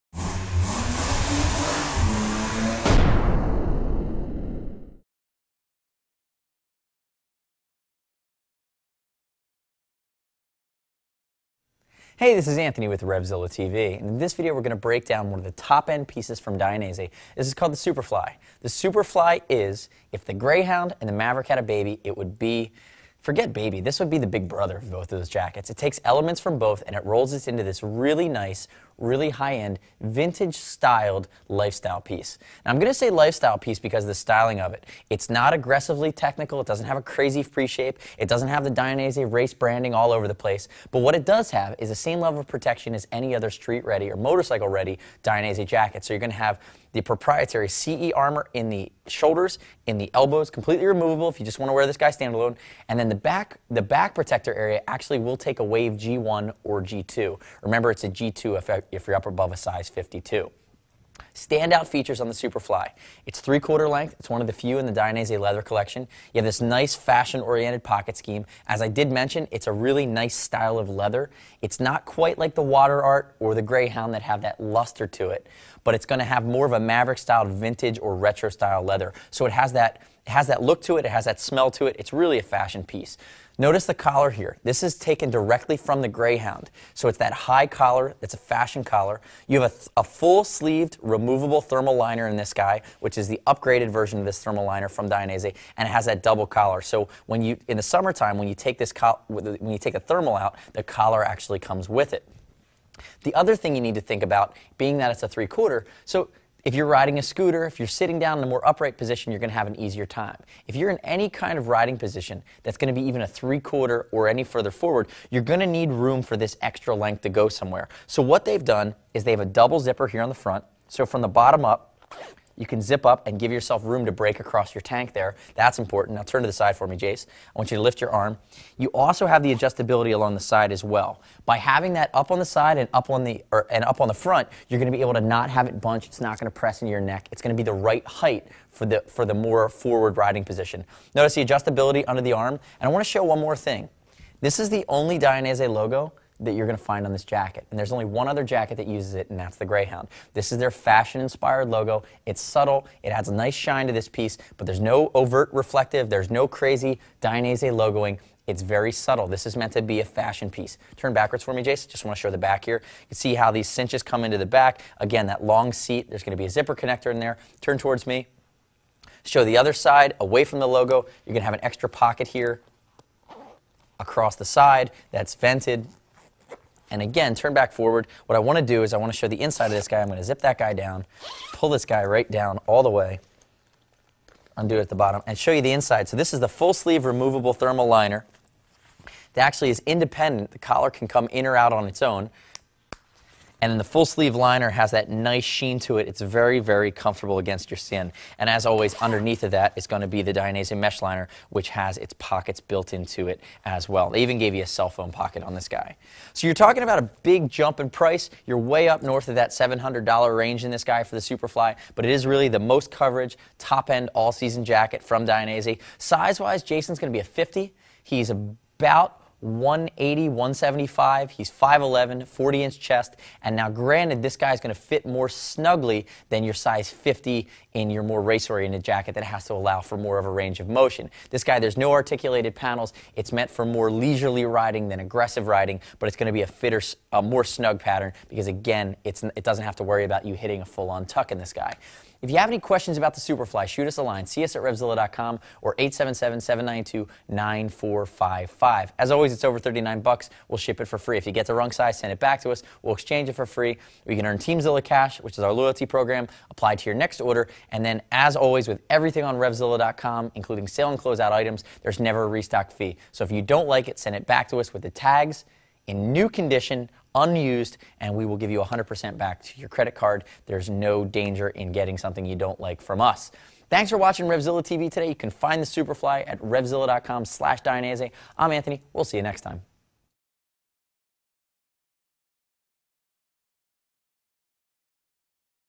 Dainese Superfly Leather Jacket Review at RevZilla.com
Dainese Superfly Leather Jacket Review
 One of the more city-styled jackets in the Dainese lineup, the Dainese Superfly Leather Jacket combines performance and protective elements with a classic vintage style that only Dainese could pull off. The resin-treated vintage leather gives this jacket an aged look that combines with the 4-pocket classic front design for a truly remarkable leather jacket. Subtle branding with just a titanium demon logo on the upper right sleeve contributes to this jacket's fashion-first mentality. There is no sacrifice in protection as there is removable CE rated armor in the shoulders and elbows and a pocket for the Wave G Back Protector. A removable microdown thermal liner keeps you warm in the cooler months while the sanitized mesh liner ensures that this jacket won't get funky. Another truly standout leather jacket from Dainese.